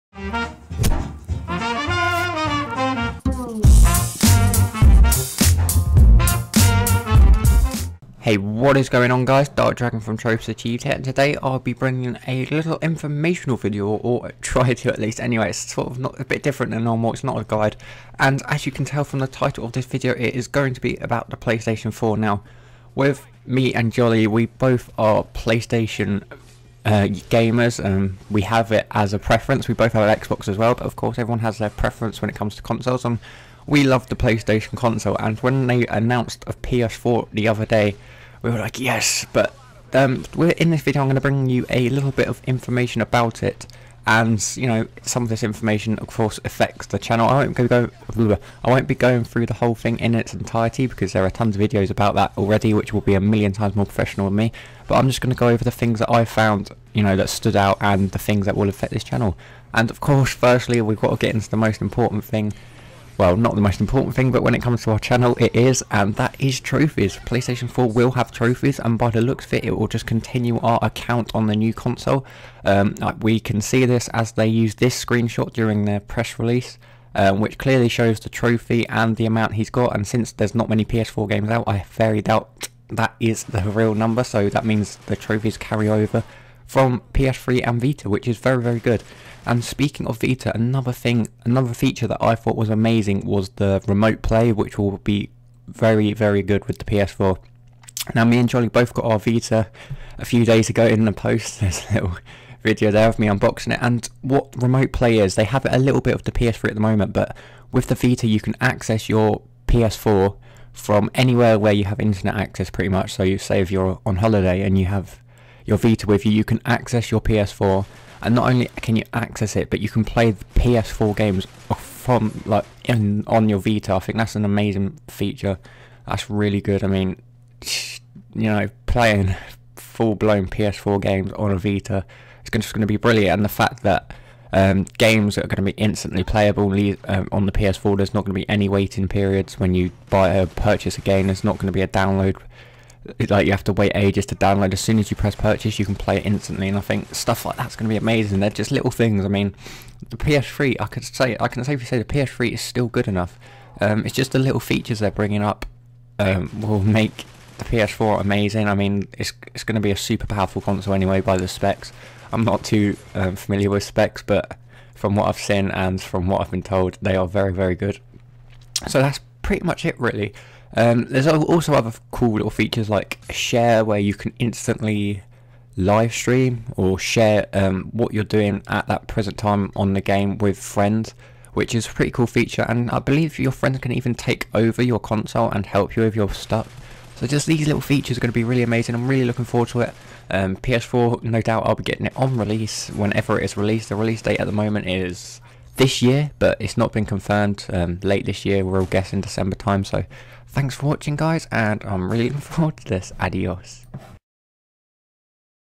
PlayStation 4 Trophies !
Hey what is going on guys, a quick little video on the cool features we noticed on the PlayStation 4 but of course the fact trophies remain means our channel lives on another generation.